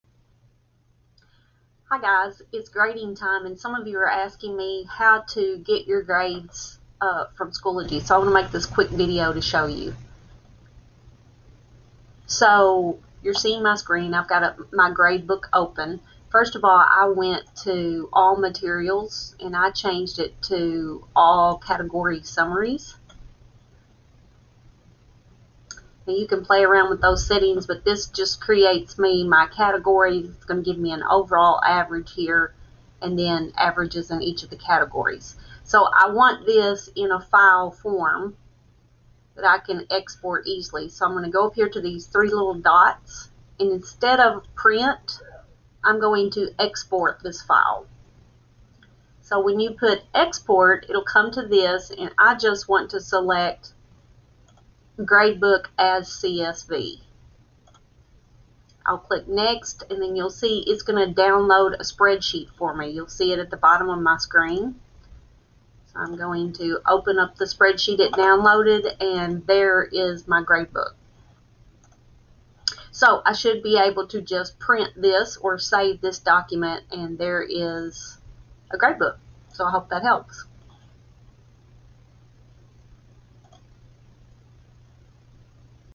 How to Print Schoology Gradebook
Needing to save or print your Schoology gradebook? Learn how to export gradebook from Schoology to save or print.